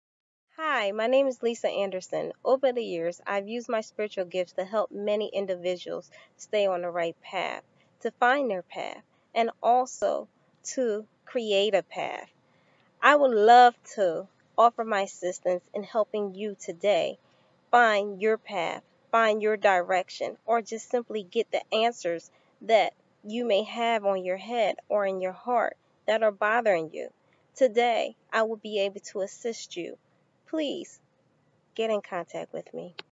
Ask The Answer - Lisa A Intro
I assisted many individuals and business owners to move their lives in a positive direction. Sometimes what holds us back is the spirit of fear, emotional bondage, spiritual and financial traps. Once you are able to know to get to the root of the problem, you will find things moving in a positive direction. I have been helping individuals get back to living a more rewarding life for years. I realized I possessed empathic and intuitive gifts as a teenager when I could pick up on what other people were feeling but never really understood the power of my gifts until my adulthood. I always liked to inspire others and could easily solve problems. My innate psychic abilities revealed answers through dreams, visions, and audio. You no longer have to feel trapped in your current position or life. By working with me, you can walk away with a plan on which direction to take in your life. I specialize in career, love, relationship and spiritual questions.